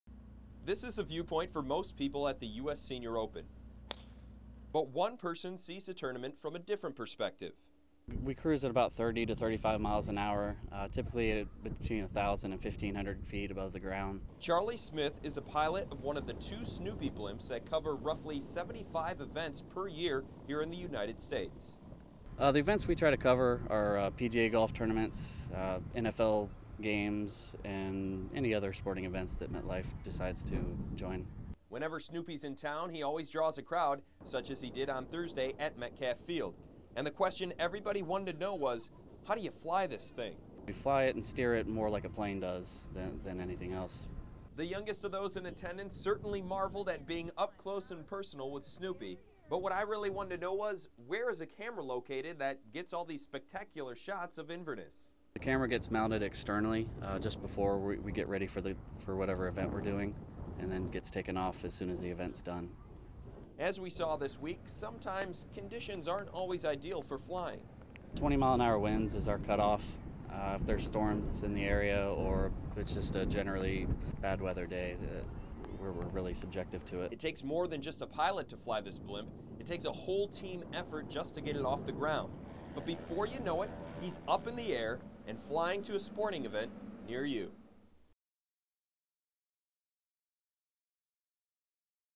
MetLife Blimp rules the skies during US Senior Open 2011
Pilot Charlie Smith saw the US Senior open tournament through a much different perspective thanks to the MetLife Snoopy blimp. The blimps cover around 75 nationwide events here in the United States.